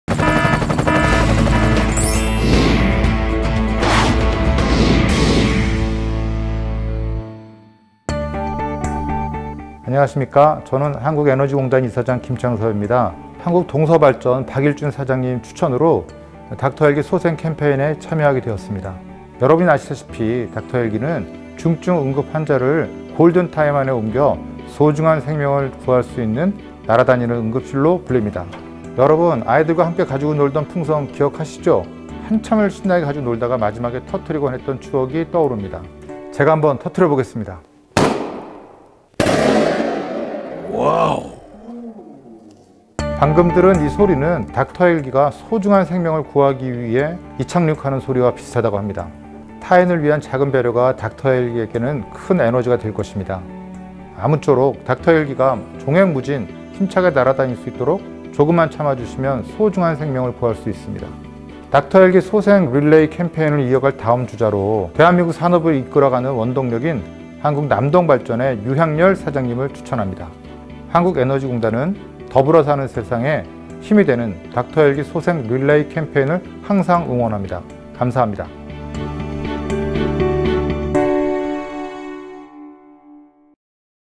한국에너지공단 닥터헬기 소생 릴레이 캠페인
한국에너지공단이 닥터헬기 소생 캠페인에 참여하게 되었습니다!

닥터헬기 소생 캠페인은 최근 소음 관련 민원 발생으로 인한 닥터헬기 운영 어려움을 알리고 닥터헬기의 중요성을 알리기 위하여 2019년 5월부터 각계 인사들이 릴레이 방식으로 진행되고 있는 캠페인입니다.